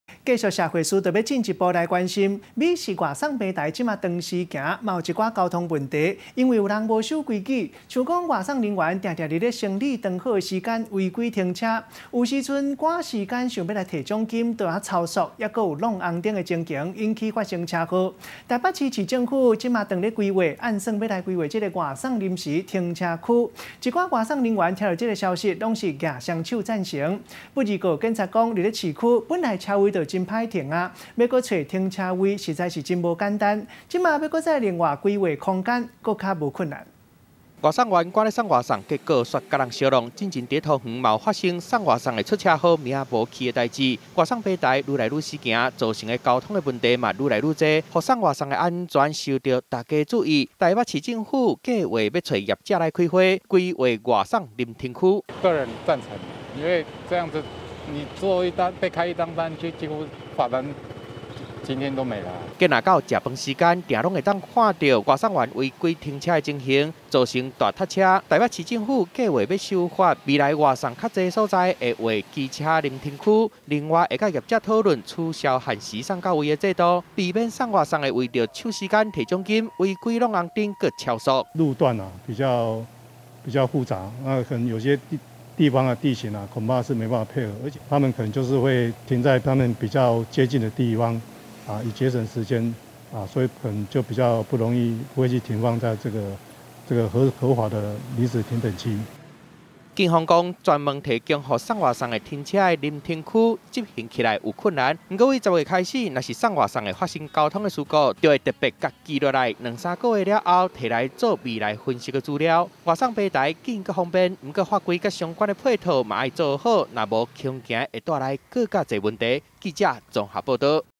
北市府擬設外送臨停區減少交通違規
美食外送平台當時行，嘛有一寡交通問題、無守規矩，像外送人員定定佇生理直好的時間，違規停車，有的趕時間想要提獎金，就超速、撞紅燈，引起發生車禍，台北市政府現在研剛，按算規劃「外送臨時停車區」，一寡外送人員聽著這e消息，攏舉雙手贊成；毋過警察講，佇市區車路，停車的車位足歹揣，欲閣另外規畫空間，實在是閣較困難。